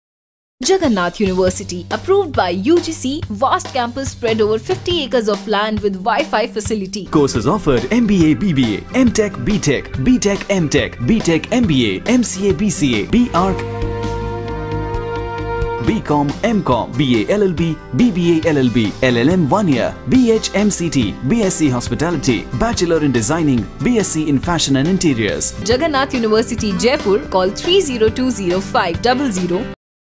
Jagannath University Spot 30 secs
Jagan Nath University has been established in the year 2008, by an Act (Act No. 19 of 2008) of the Rajasthan State Legislature and is approved under section 2(f) of UGC Act 1956 vested with the authority to award Degrees, Diplomas and Certificates. The University has been established to cater to the growing needs of higher education in the country. One of the primary objectives ...